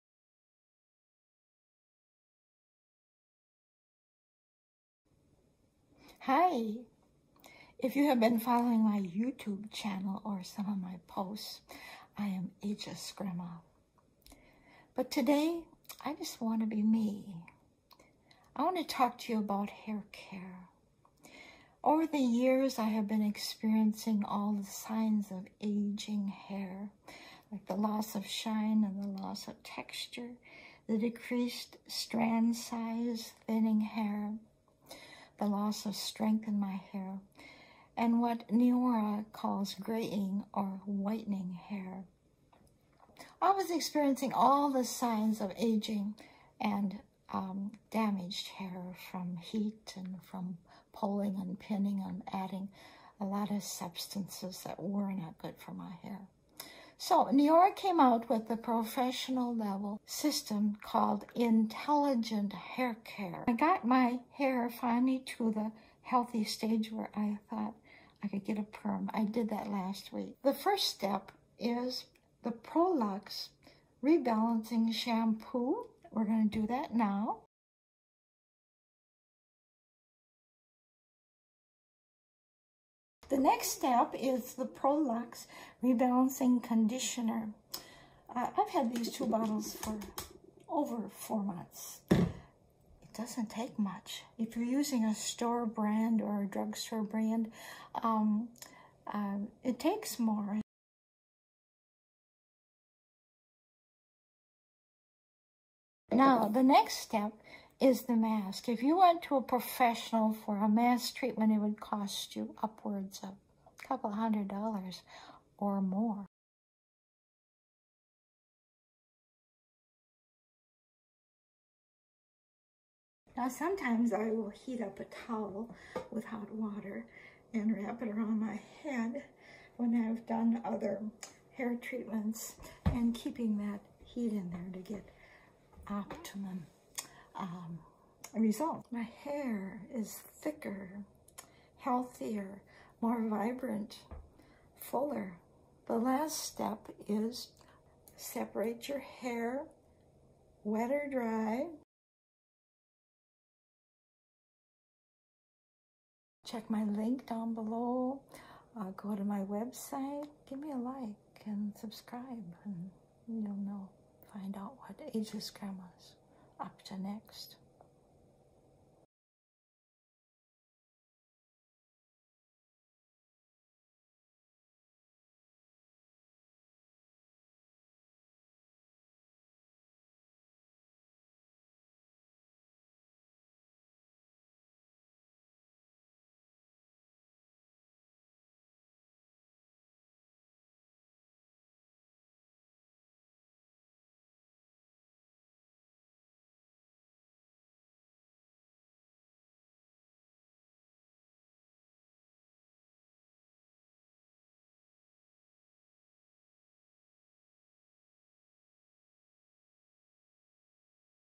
INSPIRED AG WITH INTELLIGENT HAIR CARE
Filled with ENCOURAGEMENT and INSPIRATION for an easy four step professional hair care system that intuitively seeks, finds and repairs damage to my hair from everyday stressors and especially for my Ageless Gramma "aging" hair.